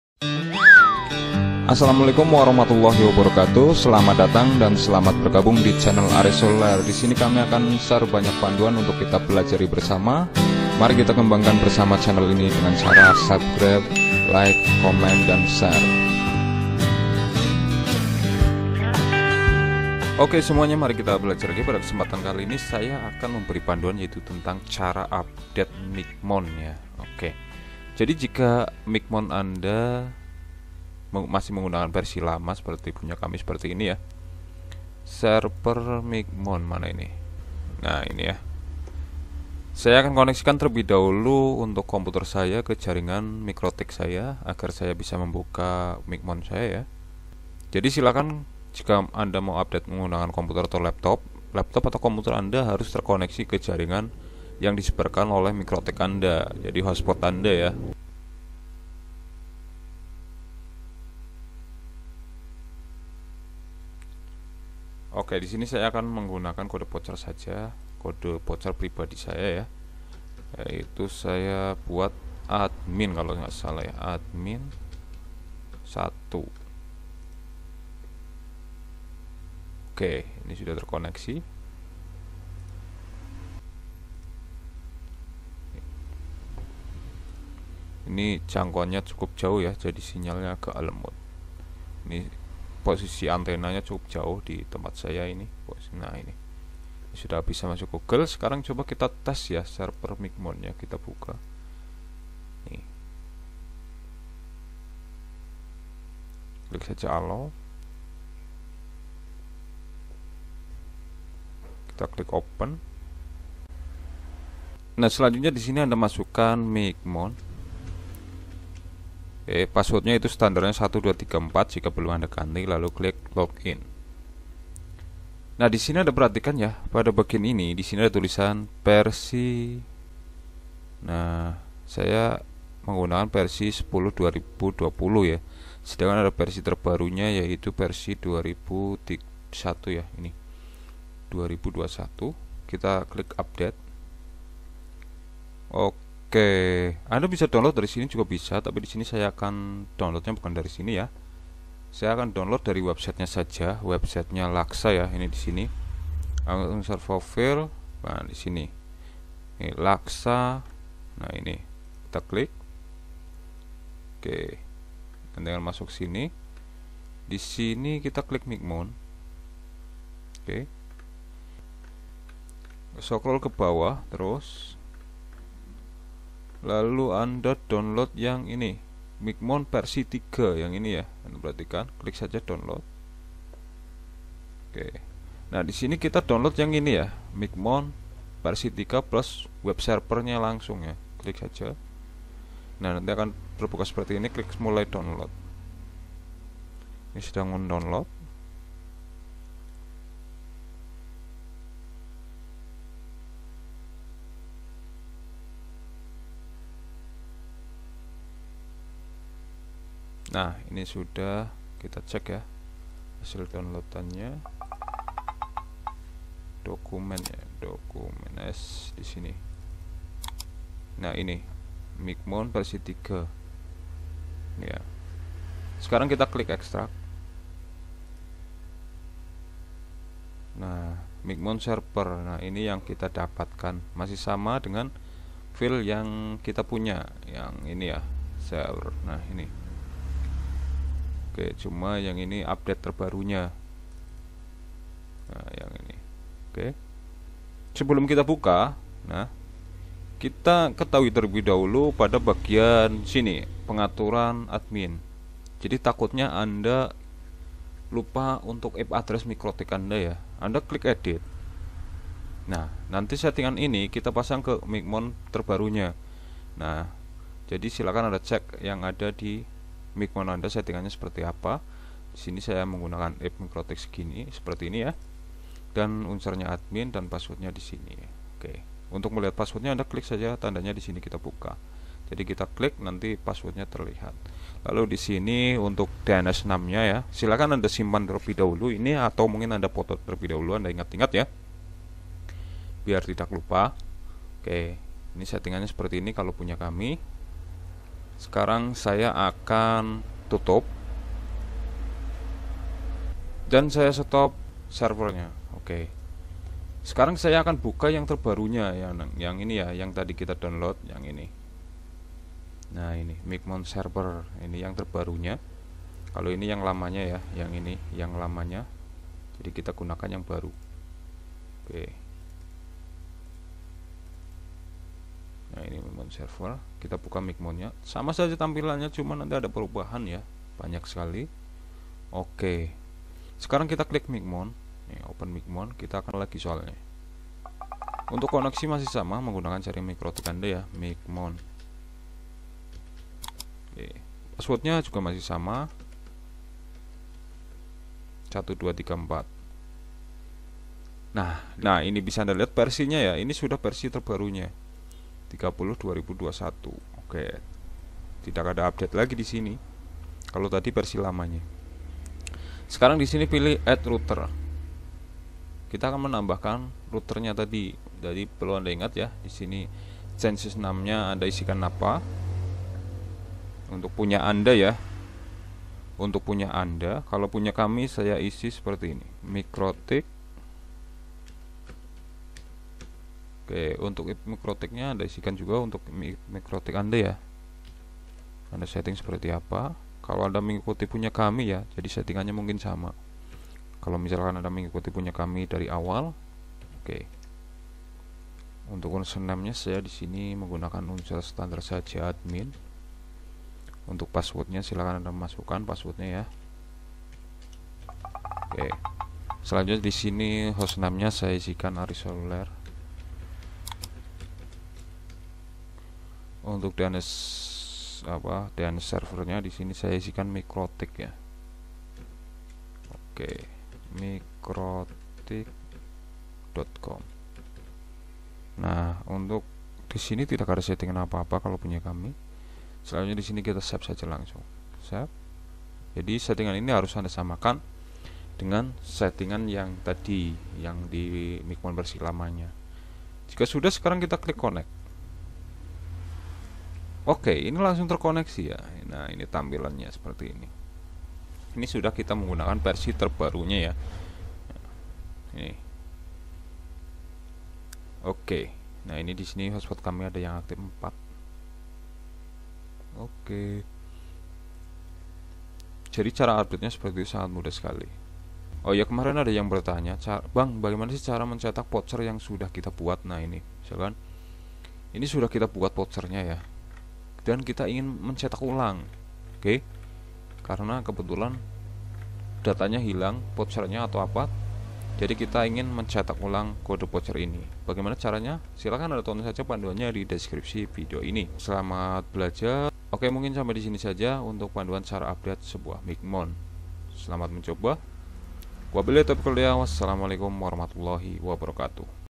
Cara update mikhmon
Assalamualaikum warahmatullahi wabarakatuh Pada kesempatan kali ini saya akan memberi panduan yaitu tentang cara update mikhmon Jika mikhmon anda masih menggunakan versi lama anda bisa update mikhmon Anda ke versi terbarunya dengan cara anda mengikuti panduan ini sudah kami buatkan Ya silakan saja cara update mikhmon ke versi terbarunya untuk mendownloadnya Anda bisa cek di bawah ini seperti contohnya membuat kode voucher mikrotik atau cara menyeting mikrotik ya dan masih banyak lagi panduannya silakan ataupun saja di bawah ini,
wabillahi taufik wal hidayah wassalamualaikum warahmatullahi wabarakatuh

Semoga bermanfaat SELAMAT BELAJAR,